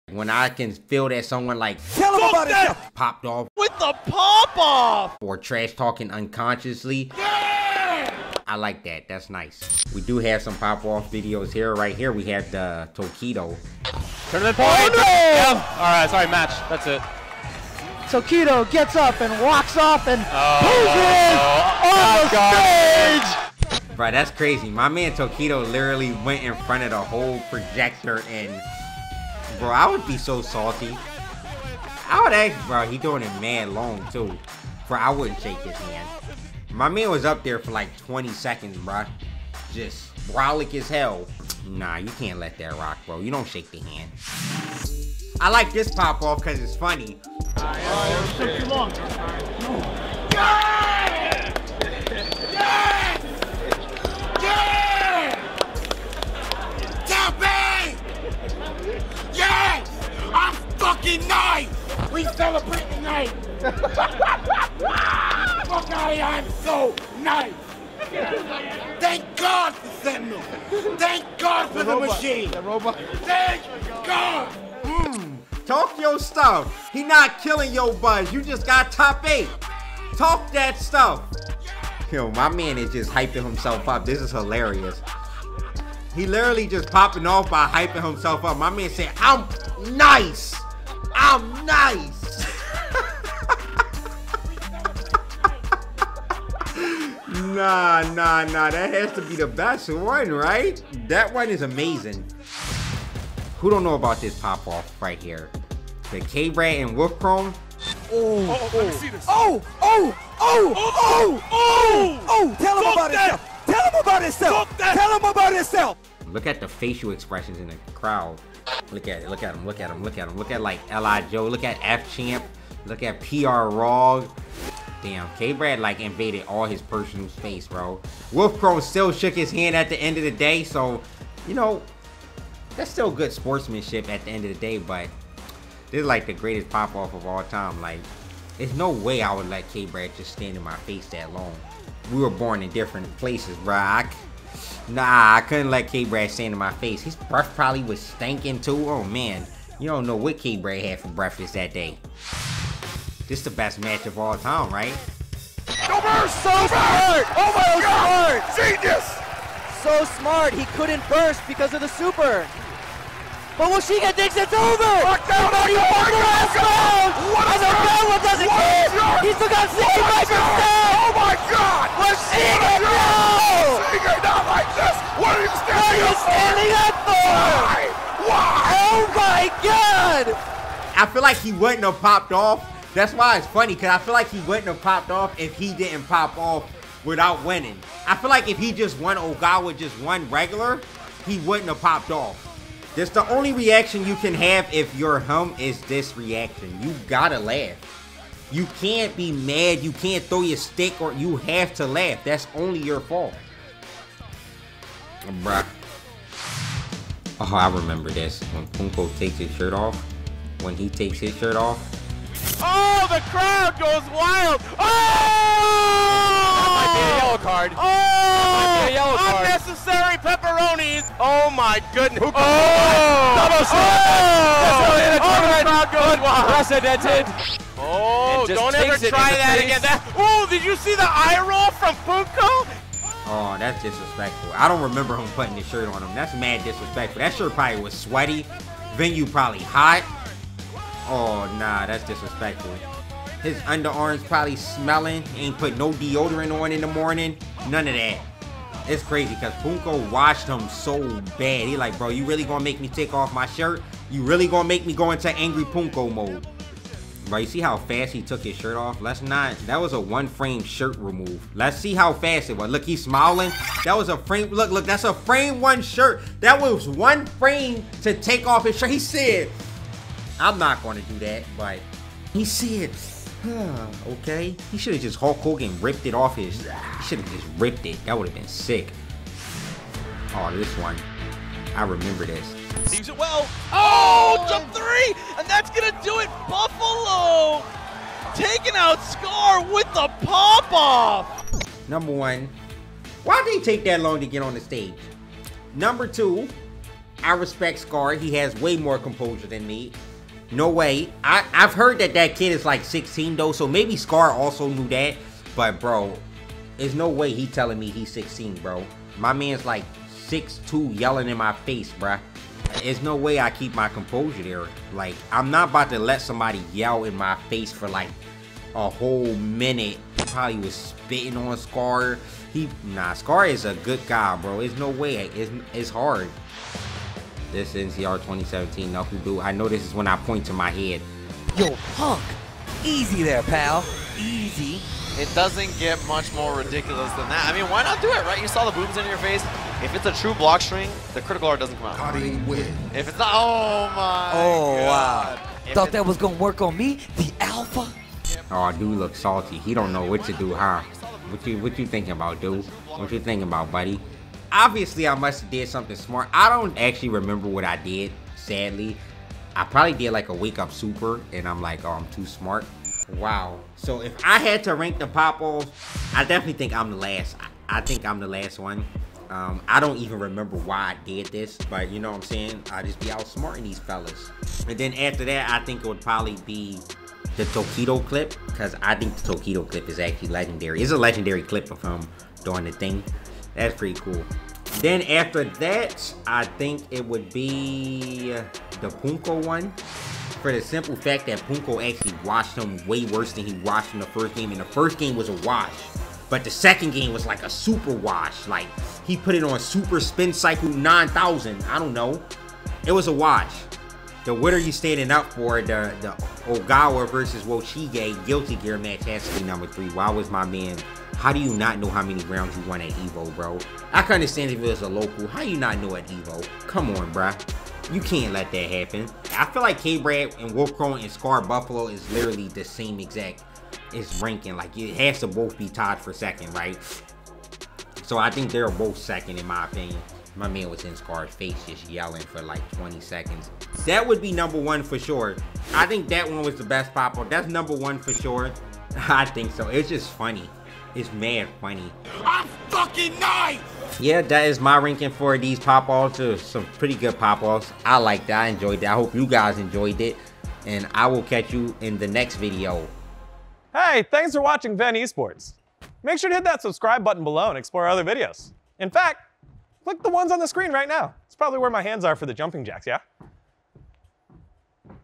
WILDEST Fighting Game Pop-Offs RANKED feat. PUNK
@Punkdagod rates the wildest moments from competitive fighting game matches. Do you agree with Punk? Which clip do you think was the craziest?

WILDEST Esports Pop-Offs RANKED feat. PUNK